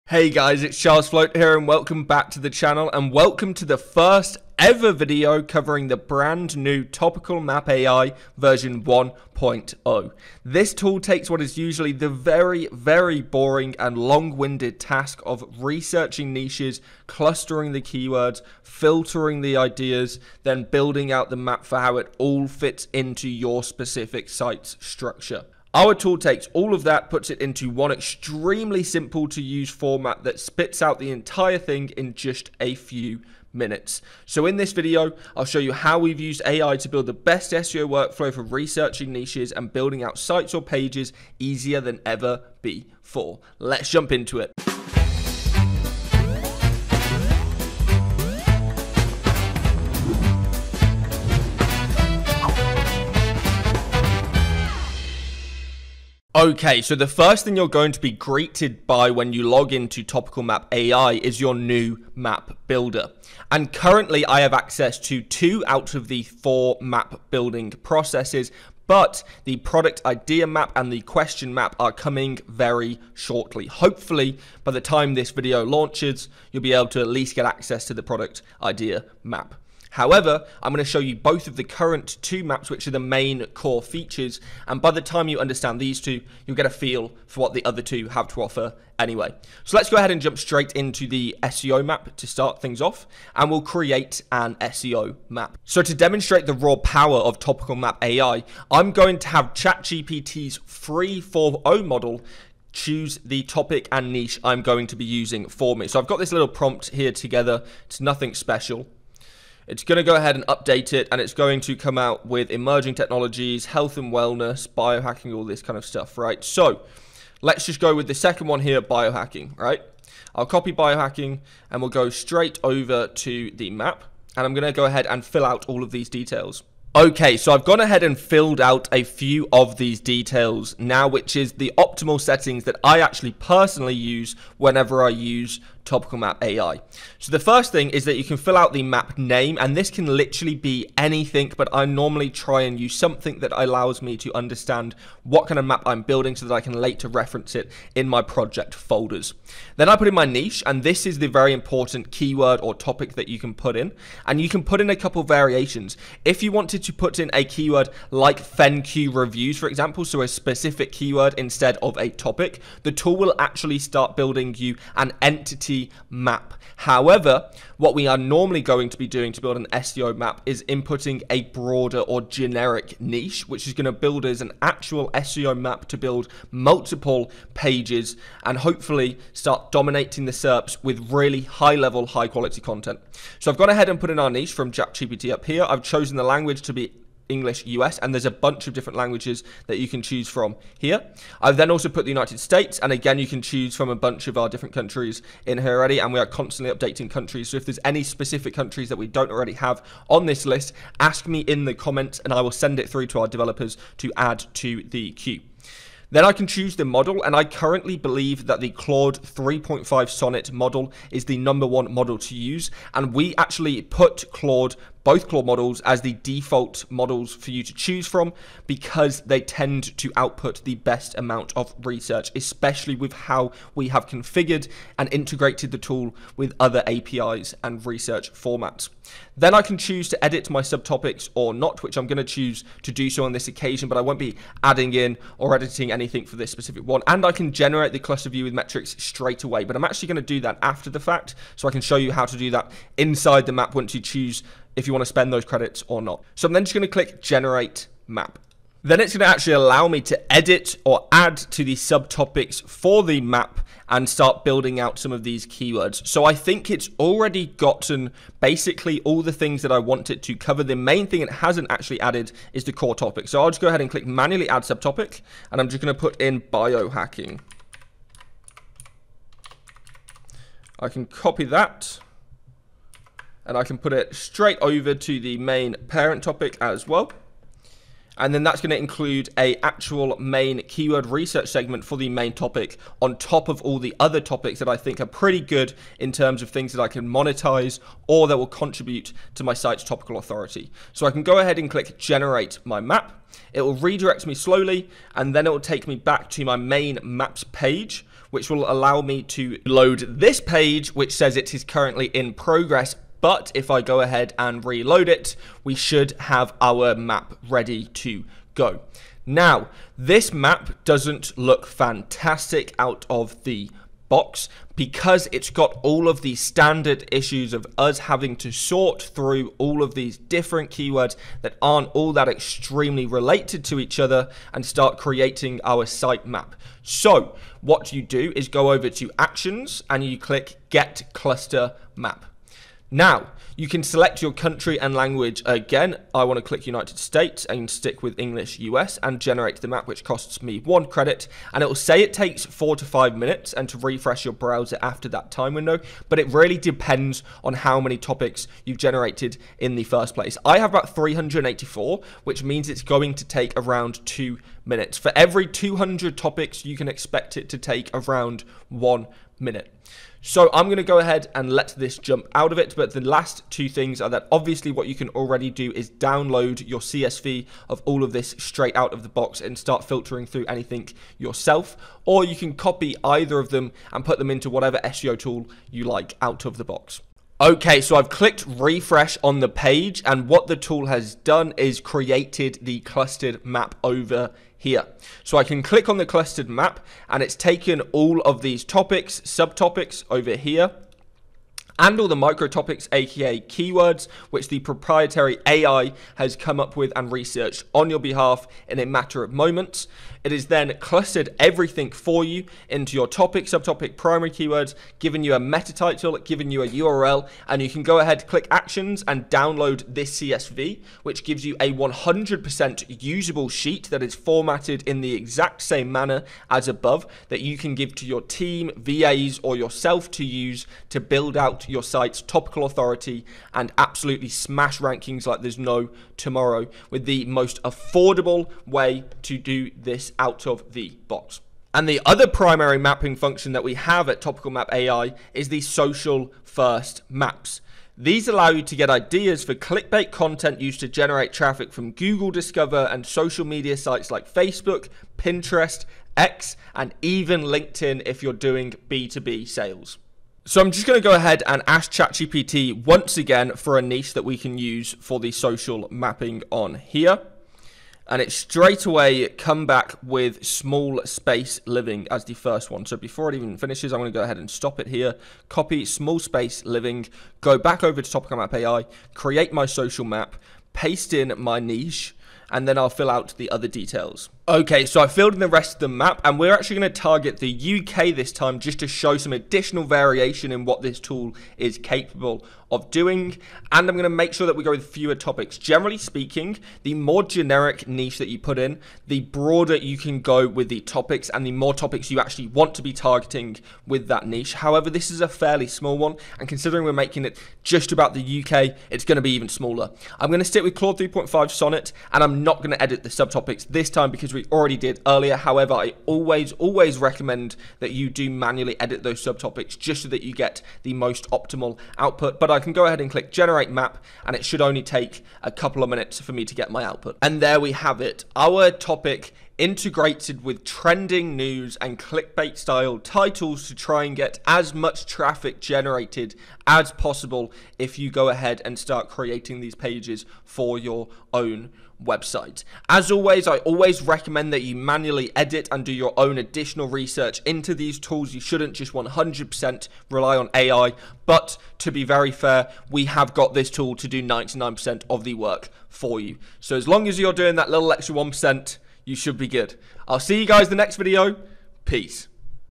How To Use Topical Map AI - TopicalMap.ai
My guide on using v1.0

Learn how to quickly build niche-specific SEO and social-first maps with the help of AI, transforming keyword research and content clustering into an efficient process. Discover the main features, usage tips, and the advanced capabilities of this tool to enhance your site's structure and dominate search engine rankings.

Timestamps:
00:00 - Introduction to Topical Map AI
00:57 - Getting Started with Your Account
01:29 - Creating an SEO Map
02:01 - Filling Out Map Details
04:51 - Generating and Editing Your Map
06:18 - Exploring the Clustered Map
07:12 - Social First Maps Overview
07:43 - Creating a Social Map
08:53 - Final Tips and Conclusion